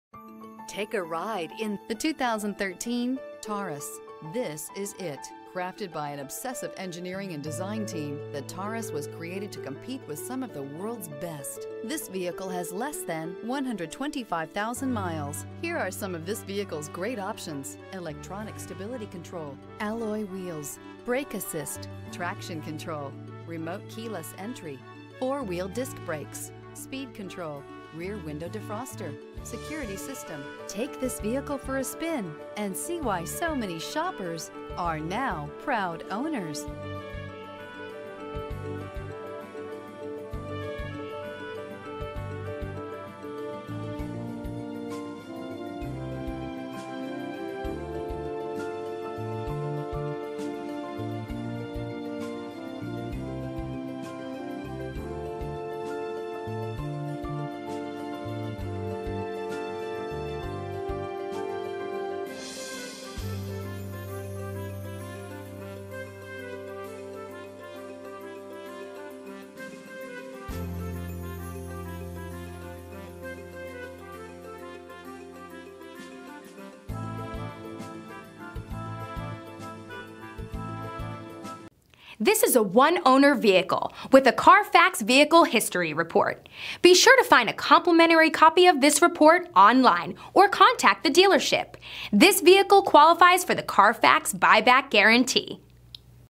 2013 Ford Taurus F3120A
Recent Arrival! 2013 Ford Taurus 3.5L 6-Cylinder SMPI DOHC Limited Platinum 19/29 City/Highway MPG Awards:br  * Ward's 10 Best Engines   * 2013 KBB.com Brand Image Awards Reviews:br  * If you like the solid feel of a full-size vehicle, but want something more modern and maneuverable than an old-fashioned SUV or wagon, the 2013 Ford Taurus is worth a test drive. Lincoln-like luxury features plus the performance-oriented SHO trim place this car in a league well above the average family sedan. Source: KBB.combr  * Long list of standard features and cutting-edge options; handsome and quiet cabin; humongous trunk; comfortable ride; available fuel-efficient four-cylinder engine; available all-wheel drive. Source: Edmunds